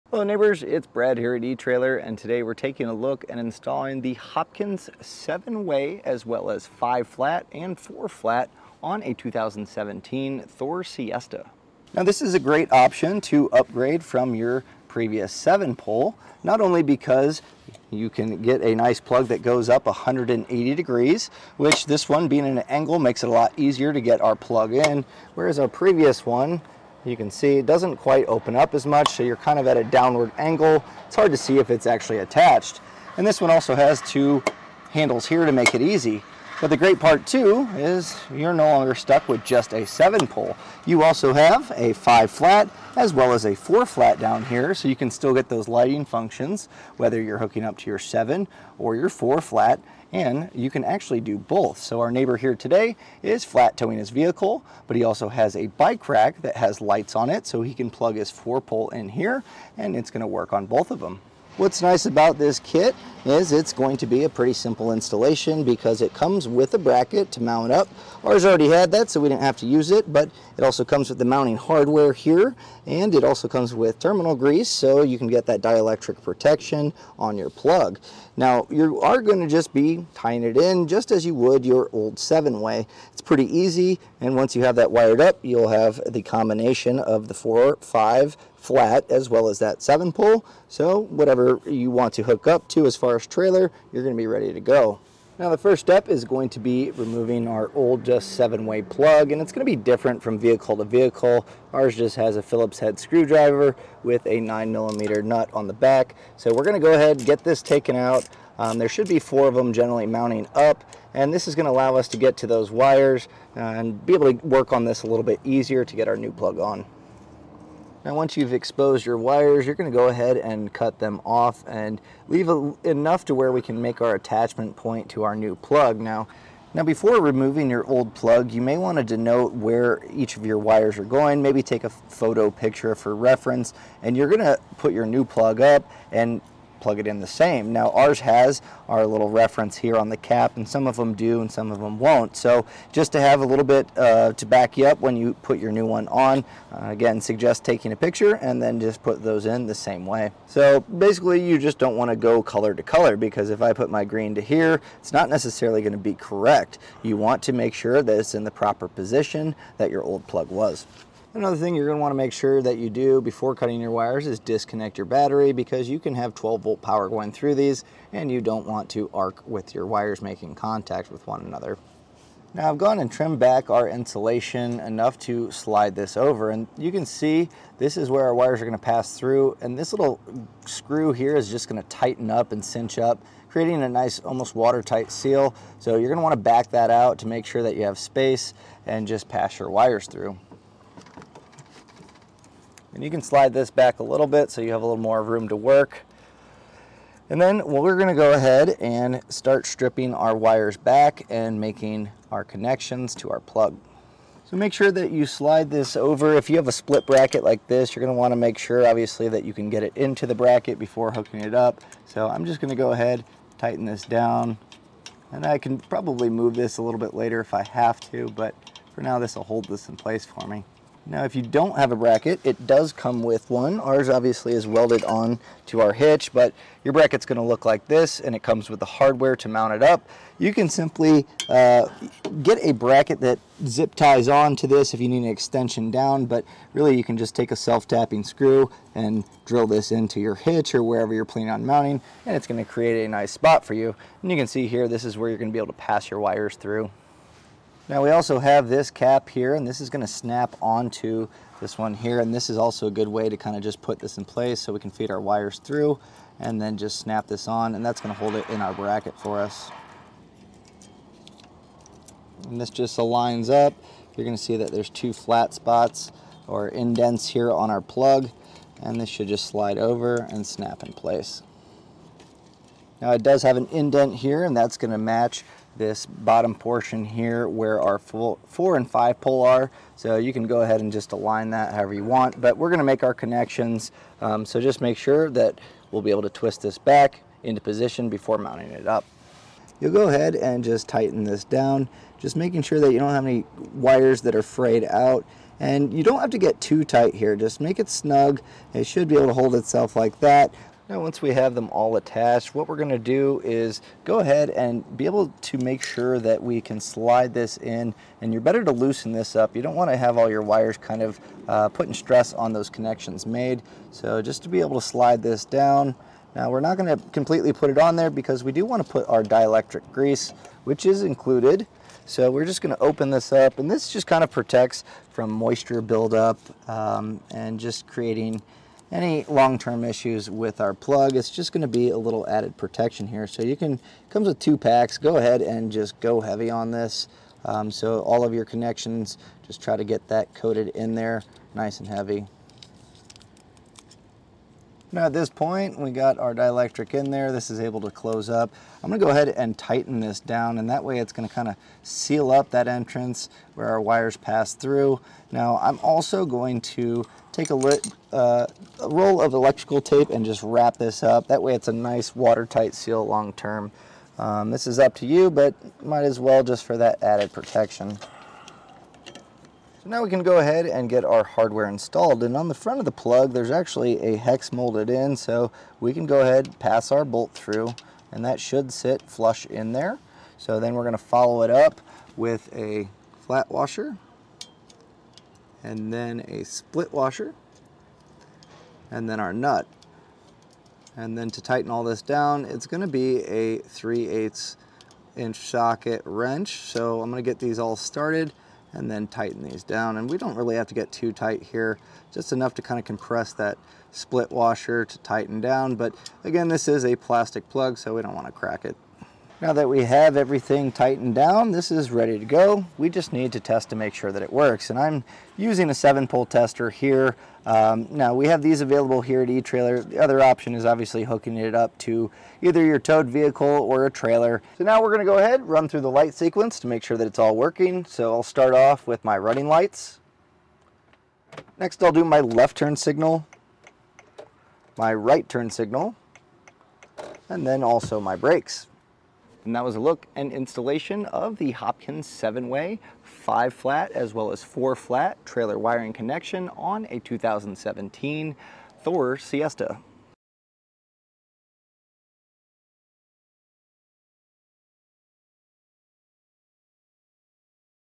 Hopkins Endurance Multi-Tow Trailer Connector Installation - 2017 Thor Siesta Sprinter Motorhome
Hello neighbors, it's Brad here at etrailer. And today we're taking a look and installing the Hopkins seven-way as well as five-flat and four-flat on a 2017 Thor Siesta. Now, this is a great option to upgrade from your previous seven-pole not only because you can get a nice plug that goes up 180 degrees, which this one being in an angle makes a lot easier to get our plug in. Whereas our previous one, you can see it doesn't quite open up as much so you're kind of at a downward angle. It's hard to see if it's actually attached. And this one also has two handles here to make it easy.

But the great part too is you're no longer stuck with just a seven pole. You also have a five flat as well as a four flat down here. So you can still get those lighting functions whether you're hooking up to your seven or your four flat and you can actually do both. So our neighbor here today is flat towing his vehicle but he also has a bike rack that has lights on it so he can plug his four pole in here and it's gonna work on both of them. What's nice about this kit is it's going to be a pretty simple installation because it comes with a bracket to mount up or has already had that so we didn't have to use it.

But it also comes with the mounting hardware here and it also comes with terminal grease. So you can get that dielectric protection on your plug. Now you are gonna just be tying it in just as you would your old seven way. It's pretty easy. And once you have that wired up, you'll have the combination of the four five flat as well as that seven pole.

So whatever you want to hook up to as far as trailer you're gonna be ready to go. Now the first step is going to be removing our old just seven way plug and it's gonna be different from vehicle to vehicle. Ours just has a Phillips head screwdriver with a nine millimeter nut on the back. So we're gonna go ahead, get this taken out. There should be four of 'em generally mounting up and this is gonna allow us to get to those wires and be able to work on this a little bit easier to get our new plug on.

Now once you've exposed your wires you're gonna go ahead and cut them off and leave enough to where we can make our attachment point to our new plug now. Now before removing your old plug you may wanna denote where each of your wires are going. Maybe take a photo picture for reference and you're gonna put your new plug there and plug it in the same. Now ours has our little reference here on the cap, and some of them do and some of 'em won't. So just to have a little bit to back you up when you put your new one on. Again, suggest taking a picture and then just put those in the same way. So basically you just don't want to go color to color because if I put my green to here, it's not necessarily gonna be correct. You want to make sure that's in the proper position that your old plug was. Another thing you're gonna wanna make sure that you do before cutting your wires is disconnect your battery because you can have 12 volt power going through these and you don't want to arc with your wires making contact with one another. Now I've gone and trimmed back our insulation enough to slide this over and you can see this is where our wires are gonna pass through. And this little screw here is just gonna tighten up and cinch up, creating a nice almost water tight seal. So you're gonna wanna back that out to make sure that you have space and just pass your wires through. And you can slide this back a little bit so you have a little more room to work. And then we're gonna go ahead and start stripping our wires back and making our connections to our plug. So make sure that you slide this over. If you have a split bracket like this you're gonna wanna make sure obviously that you can get it into the bracket before hooking it up. So I'm just gonna go ahead, tighten this down and I can probably move this a little bit later if I have to. But for now, this will hold this in place for me. Now if you don't have a bracket, it does come with one. Ours obviously is welded on to our hitch but your bracket's gonna look like this and it comes with the hardware to mount it up. You can simply get a bracket that zip ties onto this if you need an extension down. But really you can just take a self tapping screw and drill this into your hitch or wherever you're planning on mounting